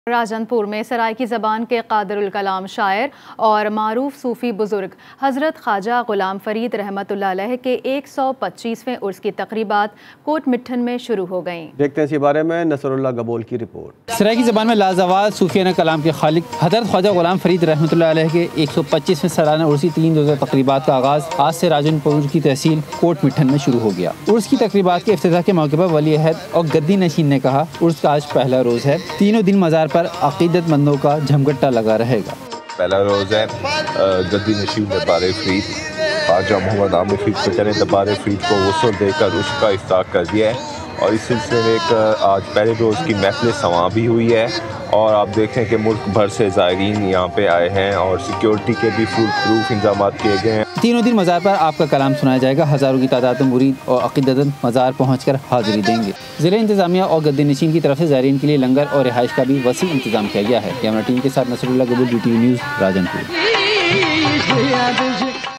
URS of Sufi Saint Khawaja Ghulam Fareed Begins in Rajanpur | Breaking News | GTV News
URS of Sufi Saint Khawaja Ghulam Fareed Begins in Rajanpur | Breaking News | GTV News | 1 Nov 2022